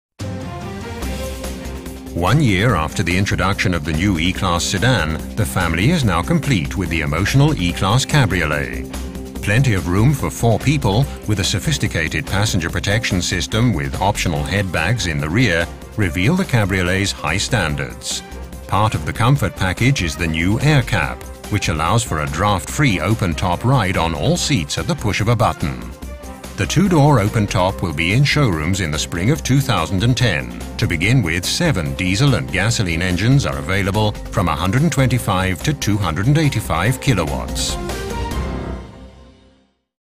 2010 Mercedes-Benz E-Class Convertible - The premiere
One year after the introduction of the new E-Class sedan the E-Class portfolio is now complete with the conception of the E-Class Convertible. Special attention was put into the development of the new draught stop AirCap. This makes wind free open top driving possible on all four seats. With the optional Headbags in the rear and invisibly integrated rollover protection the two door open top sets new standards for four seater premium convertible with a cloth top.